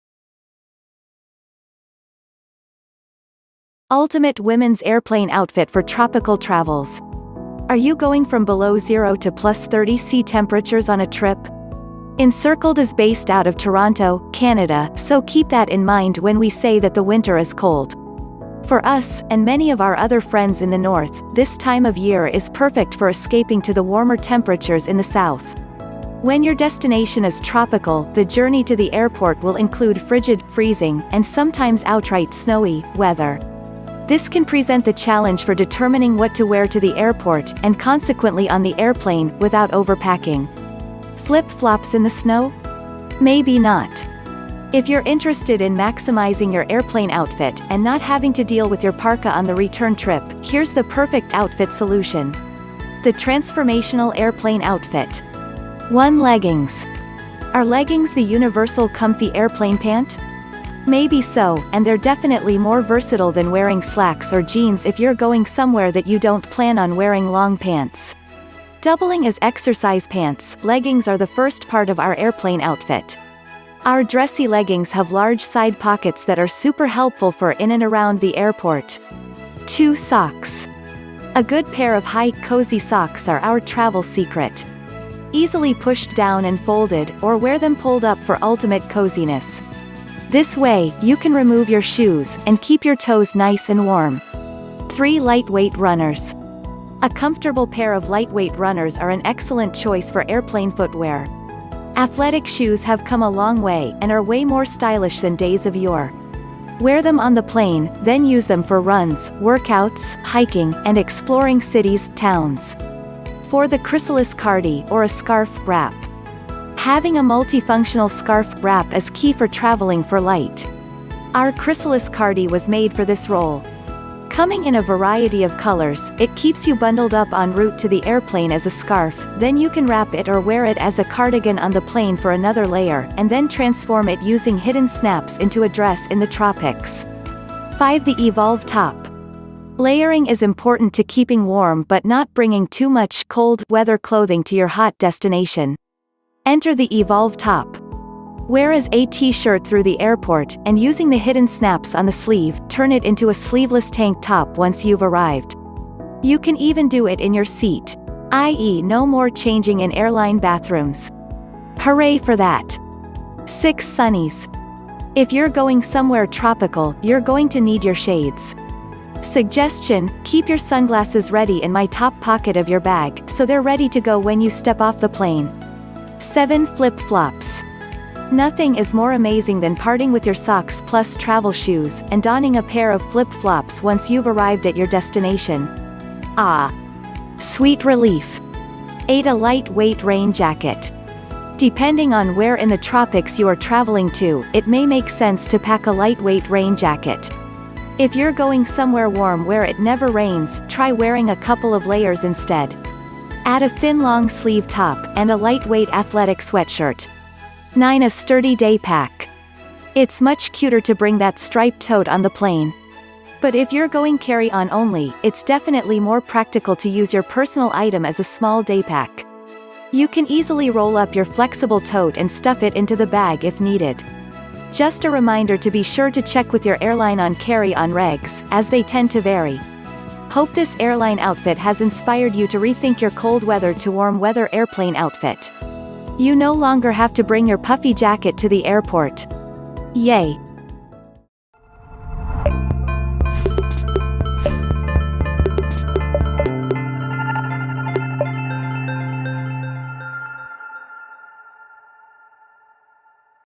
TIPS - Women's Airplane Outfit for Tropical Travels!
Ultimate Women's Airplane Outfit for Tropical Travels!

Are you going from Below Zero to Plus 30C temperatures on a trip?
Encircled is based out of Toronto, Canada, so keep that in mind when we say that  the winter is COLD. For us, and many of our other friends in the north, this time of year is perfect for escaping to the warmer temperatures in the south.
When your destination is tropical, the journey to the airport will include frigid, freezing, and sometimes outright snowy, weather. This can present the challenge for determining what to wear to the airport, and consequently on the airplane, without overpacking. Flip flops in the snow? Maybe not!
If you’re interested in maximizing your airplane outfit, and not having to deal with your parka on the return trip, here’s the perfect outfit solution.

The Transformational Airplane Outfit

1 - Leggings
Are leggings the universal comfy airplane pant? Maybe so, and they’re definitely more versatile than wearing slacks or jeans if you’re going somewhere that you don’t plan on wearing long pants! Doubling as exercise pants, leggings are the first part of our airplane outfit. Our Dressy Leggingshave large side pockets that are super helpful for in and around the airport.

2 - Socks
A good pair of high, cozy socks are our travel secret. Easily pushed down and folded, or wear them pulled up for ultimate coziness. This way, you can remove your shoes, and keep your toes nice and warm.

3 - Lightweight runners
A comfortable pair of lightweight runners are an excellent choice for airplane footwear. Athletic shoes have come a long way, and are way more stylish than days of yore.
Wear them on the plane, then use them for runs/workouts,  hiking, and exploring cities/towns.

4 - The Chrysalis Cardi (Or a Scarf/Wrap)
Having a multifunctional scarf/wrap is key for traveling for light. OurChrysalis Cardi was MADE for this role. Coming in a variety of colours, it keeps you bundled up en route to the airplane as a scarf, then you can wrap it or wear it as a cardigan on the plane for another layer, and then transform it using hidden snaps into a dress in the tropics.

5 - The Evolve Top
Layering is important to keeping warm but not bringing too much 'cold' weather clothing to your hot destination. Enter The Evolve Top. Wear as a t-shirt through the airport, and using the hidden snaps on the sleeve, turn it into a sleeveless tank top once you’ve arrived. You can even do it in your seat... i.e. no more changing in airline bathrooms. Hooray for that!

6 - Sunnies
If you're going somewhere tropical, you're going to need your shades! Suggestion: keep your sunglasses ready in my top pocket of your bag, so they’re ready to go when you step off the plane!

7 - Flip Flops
Nothing is more amazing than parting with your socks + travel shoes, and donning a pair of flip flops once you've arrived at your destination. Ahhhhh. Sweet relief!

8 - A light-weight rain jacket
Depending on where in the tropics you are traveling to, it may make sense to pack a lightweight rain jacket. If you're going somewhere warm where it NEVER rains, try wearing a couple of layers instead. Add a thin long-sleeve top, and a lightweight athletic sweatshirt.

9 - A sturdy day-pack
It's much cuter to bring that striped tote on the plane. BUT if you're going carry-on only, it's definitely more practical to use your personal item as a small daypack. You can easily roll up your flexible tote and stuff it into the bag if needed. Just a reminder to be sure to check with your airline on carry-on regs, as they tend to vary.

Hope this airline outfit has inspired you to re-think your cold weather to warm weather airplane outfit.

You no longer have to bring your puffy jacket to the airport. Yay!

Acknowledgements
Some of the images used in your video legally require you to acknowledge the image's creator. An easy way to do this is to copy and paste the message below into your video description or blog post. Learn more.